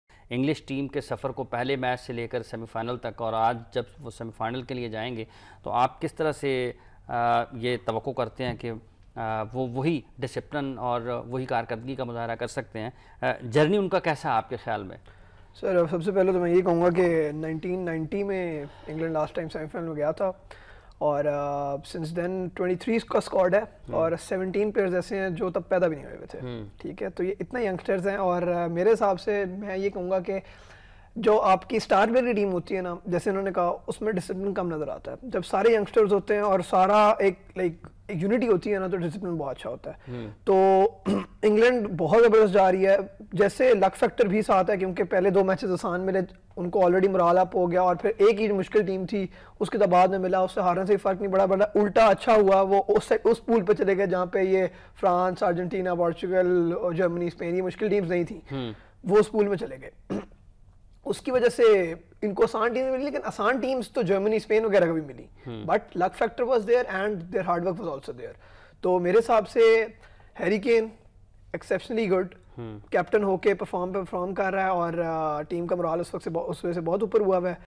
آج جب انگلینڈ ٹیم نے سیمی فائنل میں رسائی حاصل کرلی.تو کیا. آگے بے اس ڈسپلین کا ساتھ رہے گی?
Program:Packages.